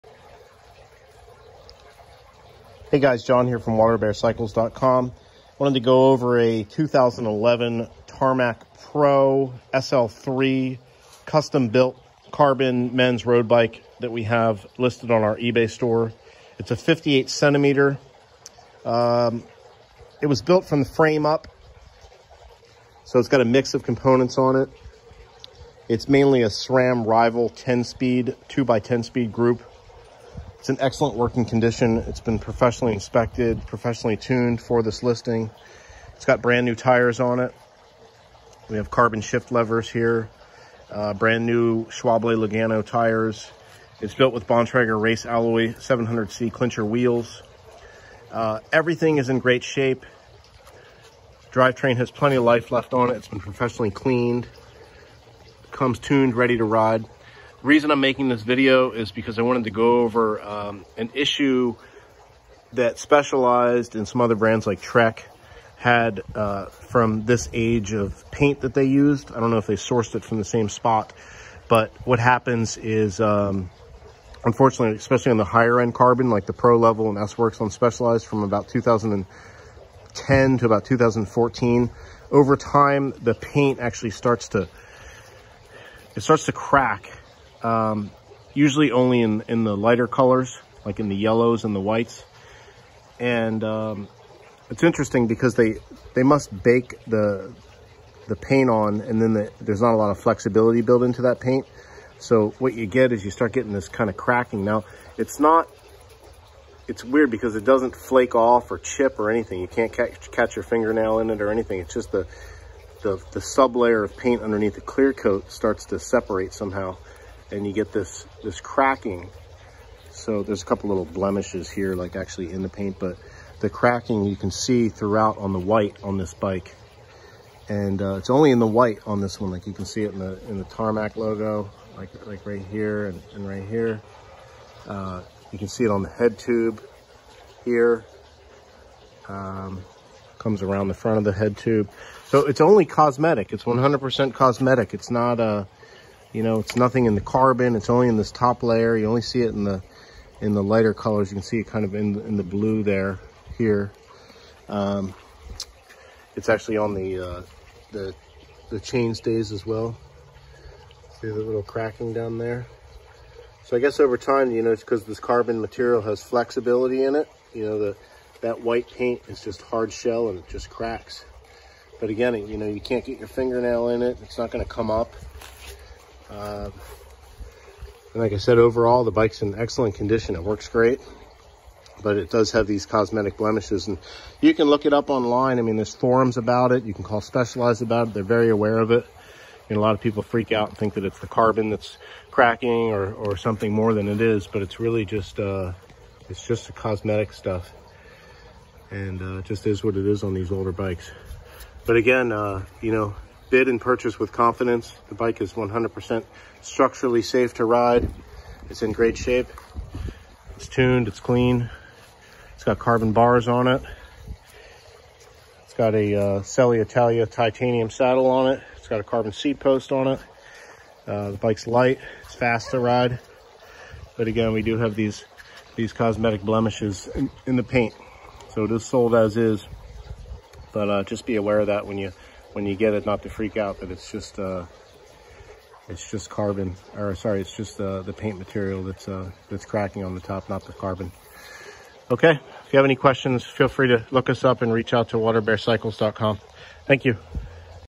Specialized Tarmac Pro Paint Blemish Issue, NOT in Carbon
This video addresses is a known issue with older paint on some high-end carbon road bicycles. The hard outer shell of paint has a tendency to crack overtime. People often get these paint cracks confused with the carbon frames being cracked, however this is not the case.